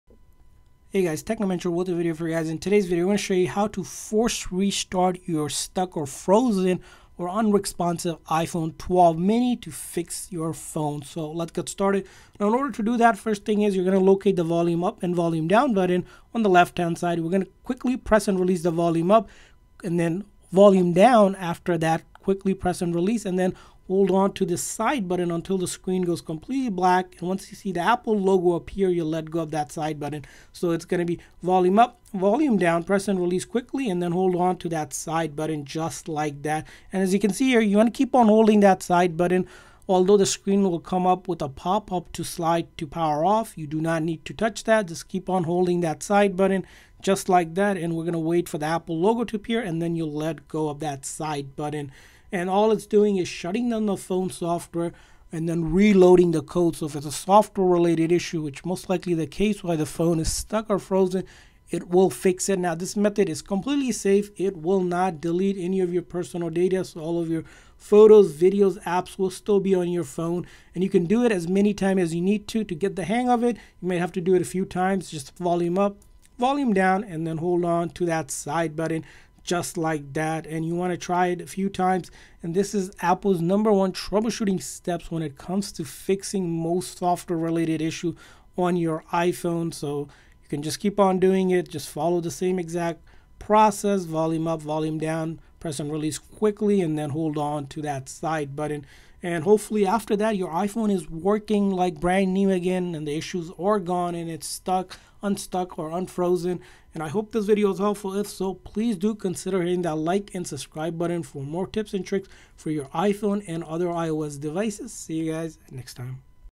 How to Force Restart a Stuck or Frozen iPhone 12 Mini
Having problems with your iPhone 12 Mini screen being frozen or stuck? To fix this problem, you will need to force restart your iPhone 12 Mini to unfreeze it or get it unstuck. In this video we show you how to force restart a stuck or frozen iPhone 12 Mini even if the touch screen is not working or responding to touch.